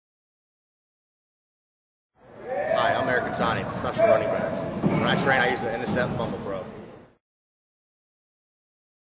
Eric Kettani for Fumble Pro
Pro running back Eric Kettani trains with the Fumble Pro.

Fumble Pro available at FumblePro.com